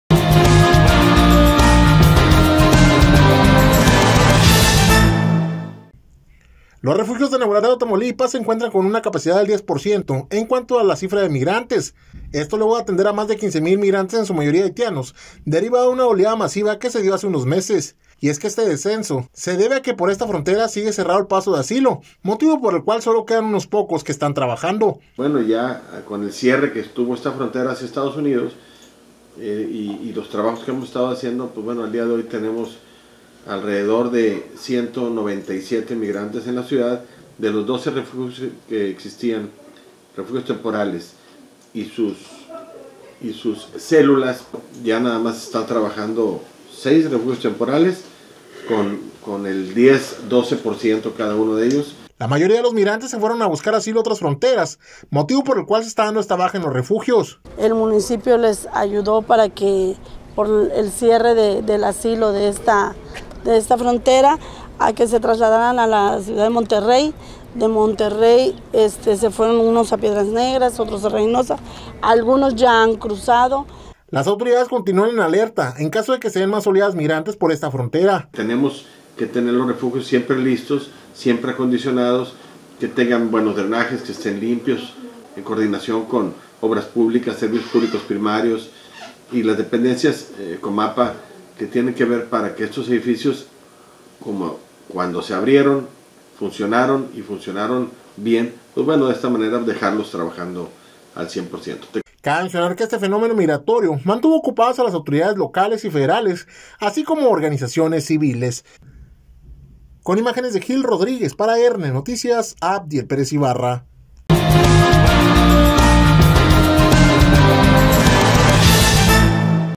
Refugios al 10 por ciento de capacidad en cuanto a migrantes RN
Refugios al 10 por ciento de capacidad en cuanto a migrantes

Los refugios de Nuevo Laredo Tamaulipas se encuentran con un capacidad del 10 por ciento en cuanto a migrantes esto luego de atender a más de 15 mil migrantes en su mayoría haitianos, derivado de la un oleada masiva que se dio hace unos meses